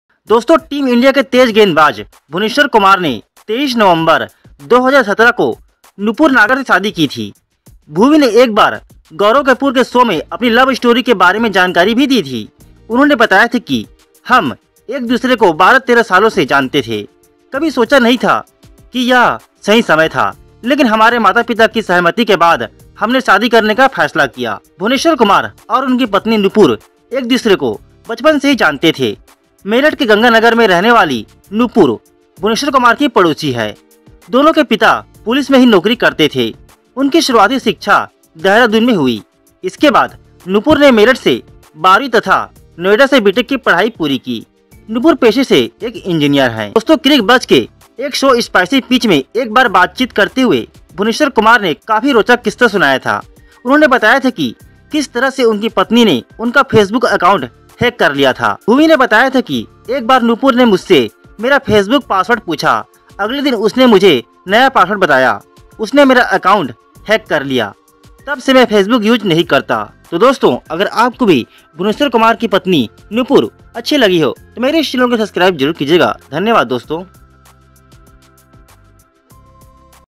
बेइंतेहा खूबसूरत हैं भुवनेश्‍वर कुमार की पत्‍नी नुपुर, पति को फेसबुक छोड़ने के लिए ऐसे कर दिया था 'म
बेइंतेहा खूबसूरत हैं भुवनेश्‍वर कुमार की पत्‍नी नुपुर, पति को फेसबुक छोड़ने के लिए ऐसे कर दिया था 'मजबूर'